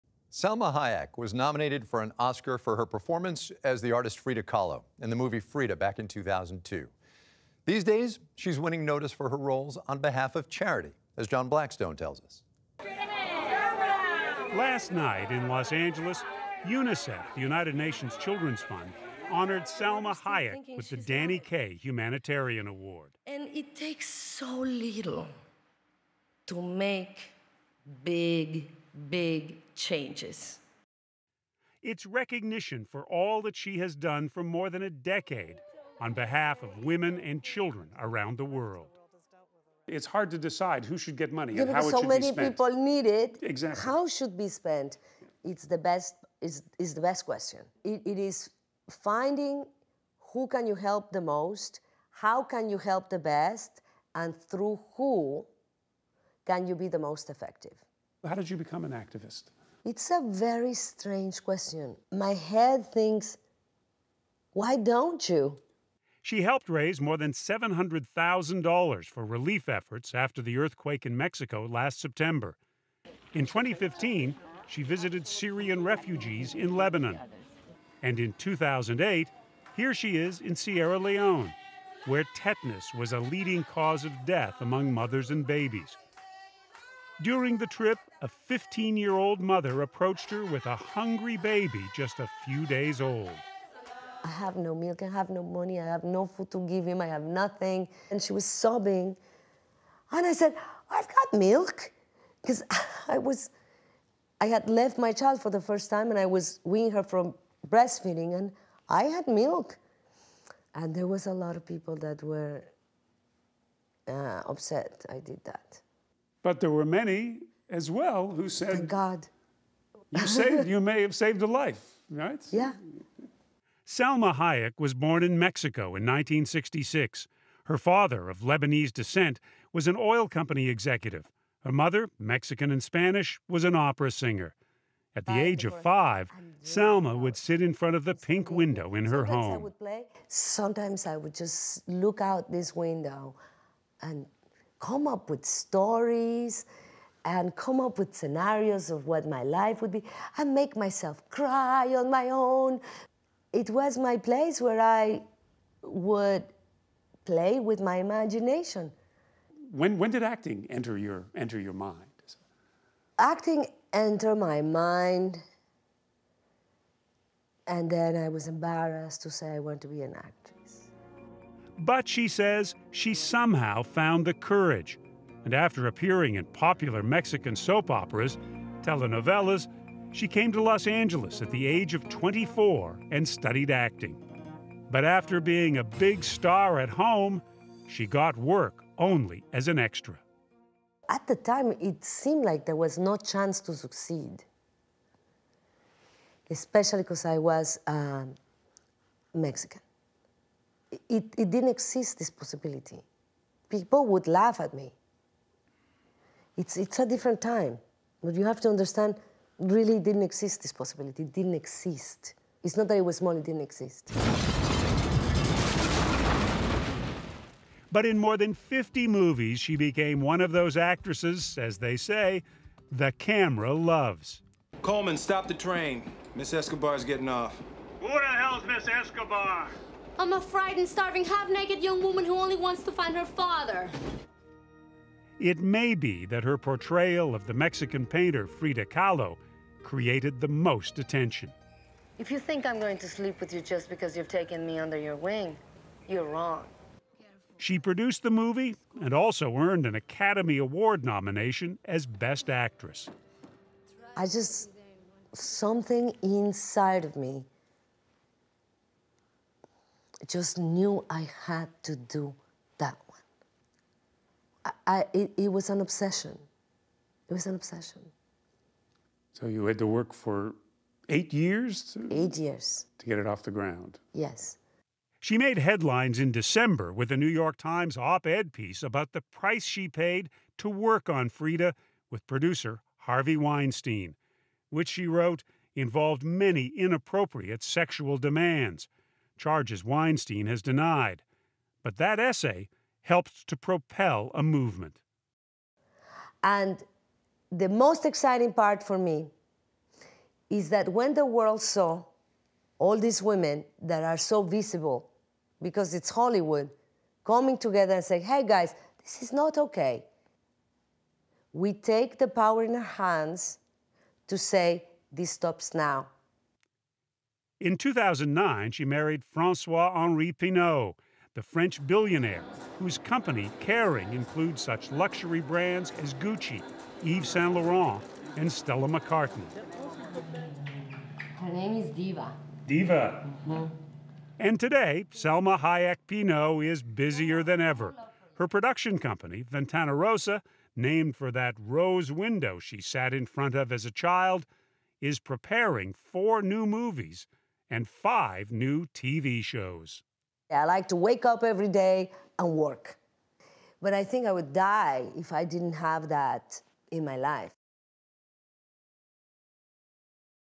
Salma Hayek Pinault talks charity, activism
The actress-producer has helped propel humanitarian efforts with her philanthropic work, and helped propel the MeToo movement with her personal stories. John Blackstone reports.